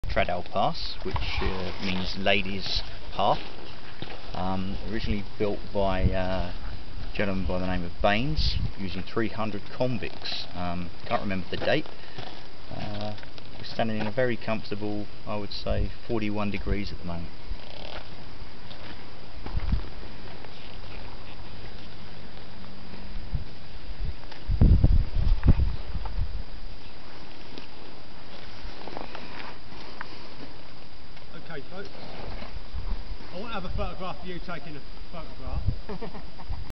Kaapstad Motorcycle Adventure Tours
A brief stop at the top of a mountian pass. Day 4..